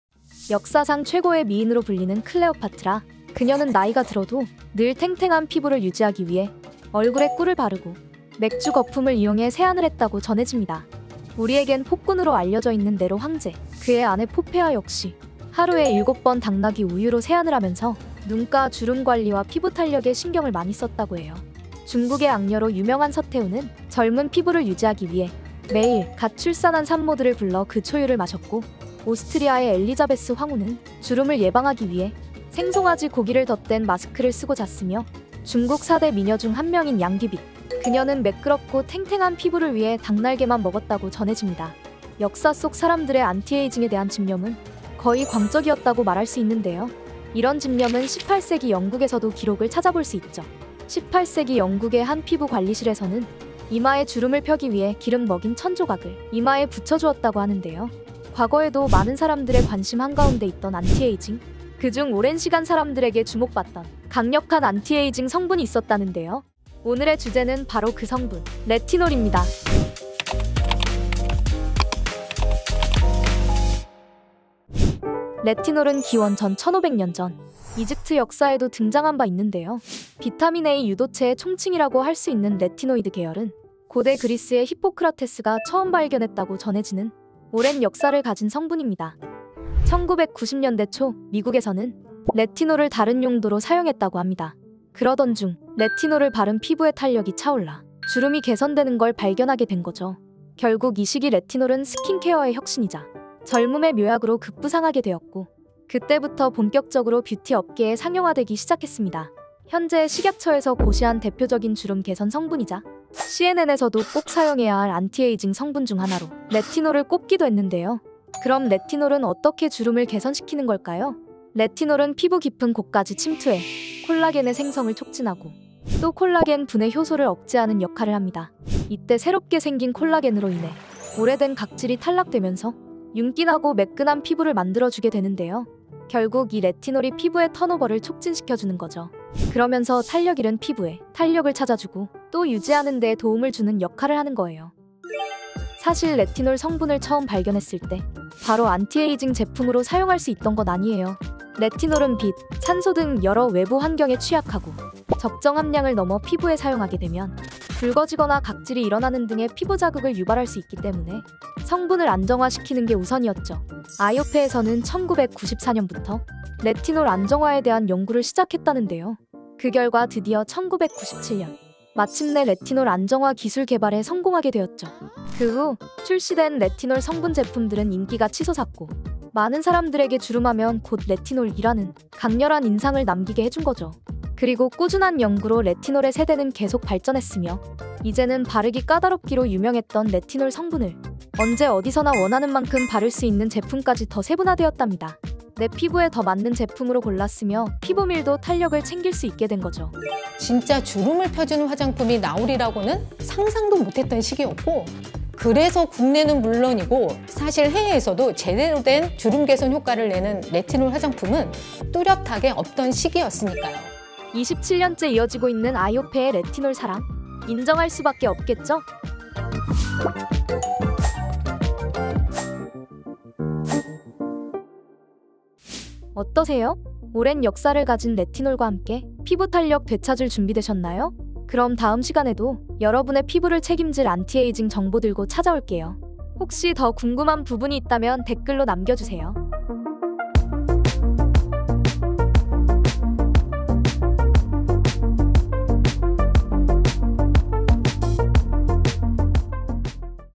옛날 사람들이 피부 노화를 막기 위해 한 '이것'?! | 피부위키🔍
옛날부터 우리 인류는 모두 피부탄력에 집착해왔죠😶‍🌫️
그렇게 세월이 흘러 인류는 궁극의 안티에이징 성분!
레티놀을 만나게 되었는데요🧑‍🔬
그 레티놀에 대해 오늘 피부위키에서 알려드릴게요💙

01:15 레티놀이 뭐죠?🤔
02:37 레티놀 발전사📚
03:46 피부위키 추천템🛍️
03:52 레티놀과 함께 피부탄력 되찾자!🙌